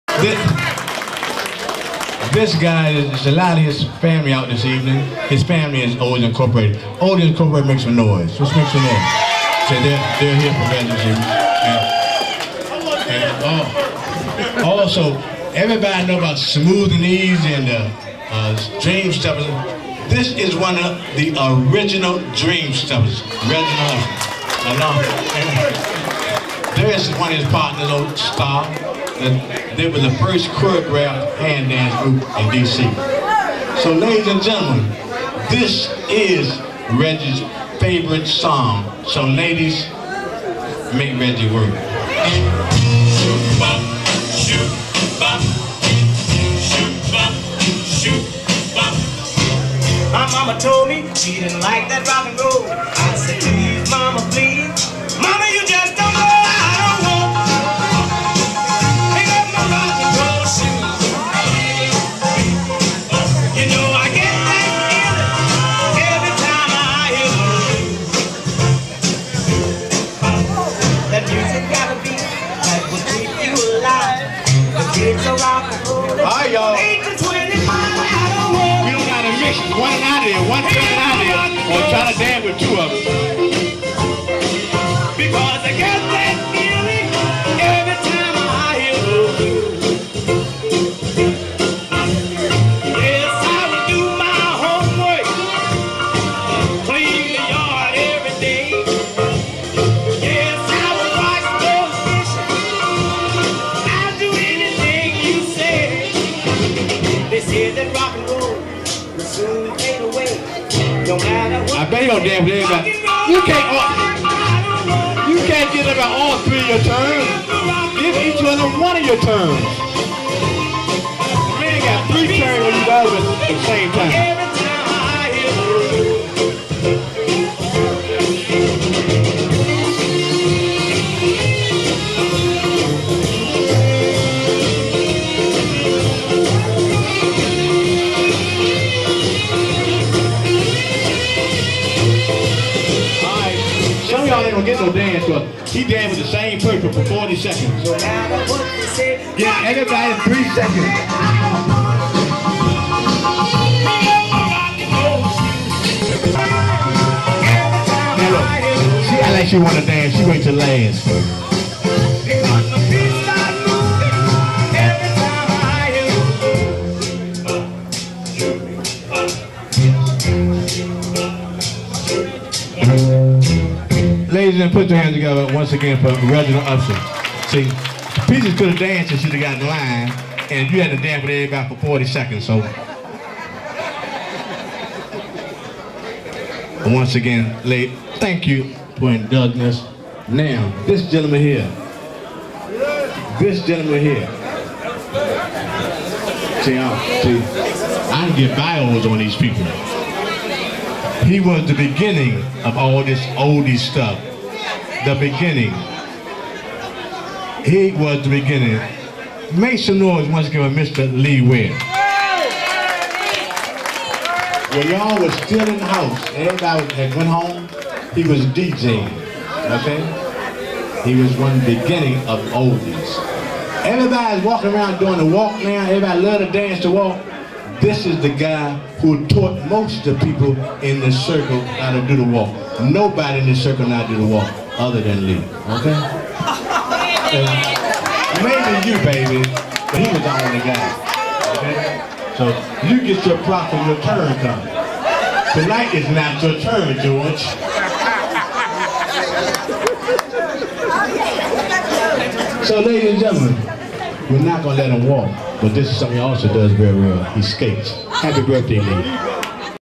IN MEMORY OF REGGIE UPSHUR
Reggie formerly of the Dream Steppers
birthday dance. MJ's Metting Place. 2006